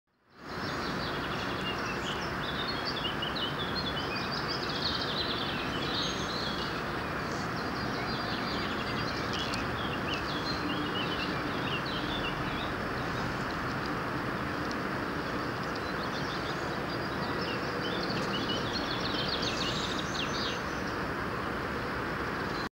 Gammares
Nuées de gammares sur la Basse rivière d'Ain